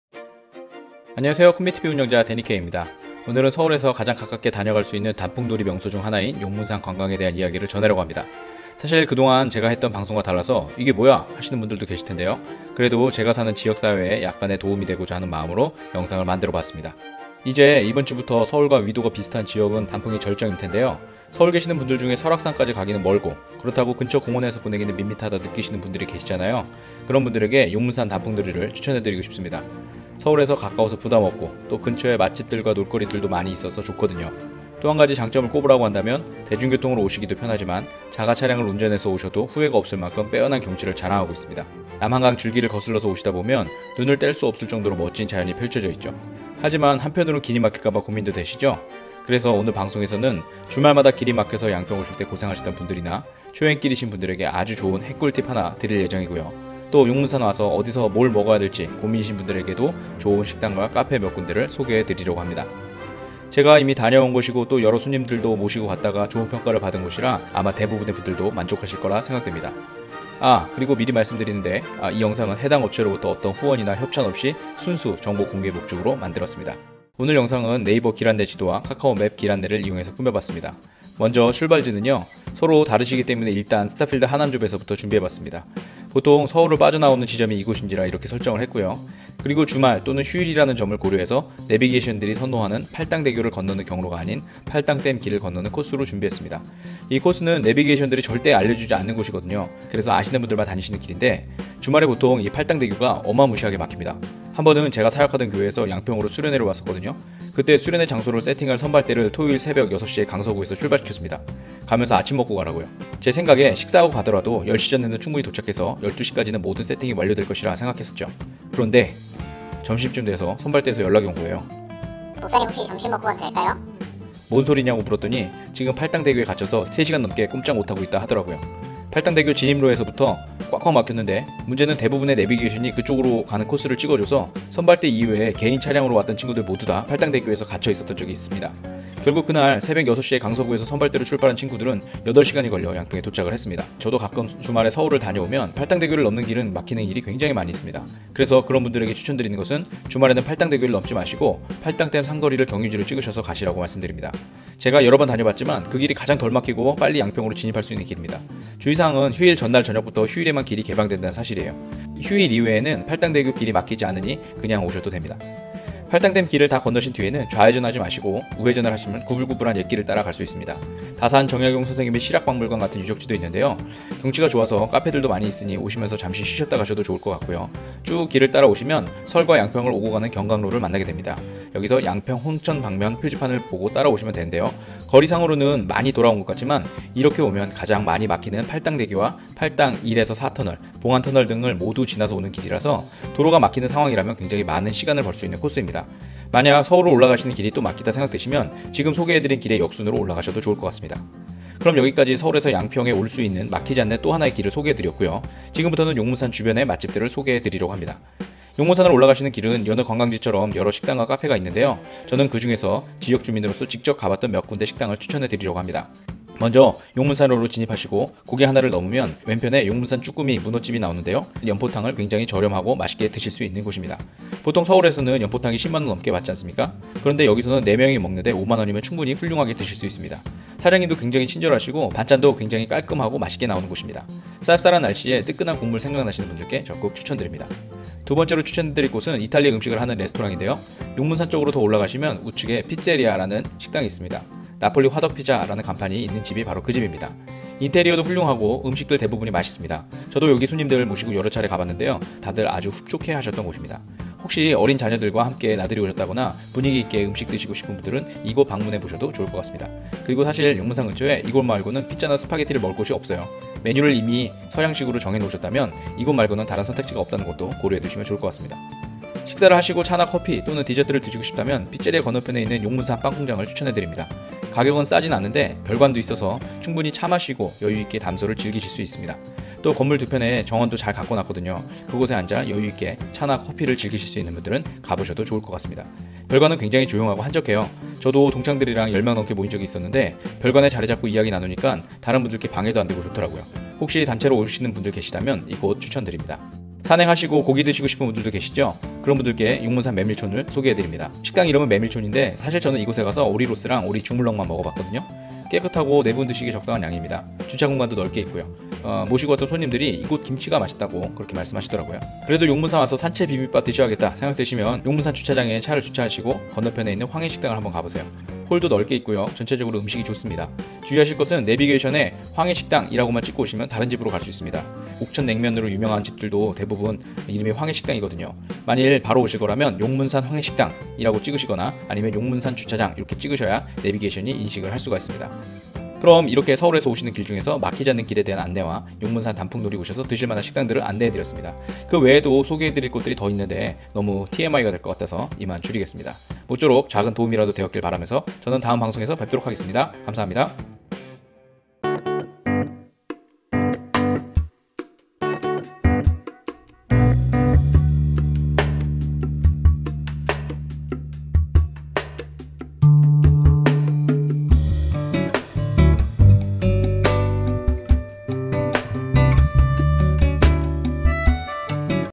용문산 맛집 4곳, 안막히는 길 소개!! - 서울 근교 단풍놀이 오실 분들을 위한 핵꿀팁(내비 언니도 모르는 길)
안녕하세요? 콘미티비 운영자 Danny K 입니다.

오늘은 서울에서 가장 가깝게 다녀갈 수 있는 단풍놀이 명소 중 하나인 용문산 관광에 대한 이야기를 전하려고 합니다.

이미 용문산 단풍 좋은 건 아시는데 한 편으론 길이 막힐까봐 고민도 되시지요?
그래서 오늘 방송에서는, 주말마다 길이 막혀서 양평 오실 때 고생하셨던 분들이나, 초행길이신 분들에게 아주 좋은 핵꿀팁 하나 드릴 예정이고요, 또 용문산 와서 어디서 뭘 먹어야 할지 고민이신 분들에게도 좋은 식당과 카페 몇 군데를 소개해드리려고 합니다.

제가 이미 다녀온 곳이고, 또 여러 손님들도 모시고 갔다가 좋은 평가를 받은 곳이라 아마 대부분의 분들도 만족하실 거라 생각됩니다.

“구독은 필수! 좋아요는 의리! 댓글은 사랑! 공유는 마음대로!”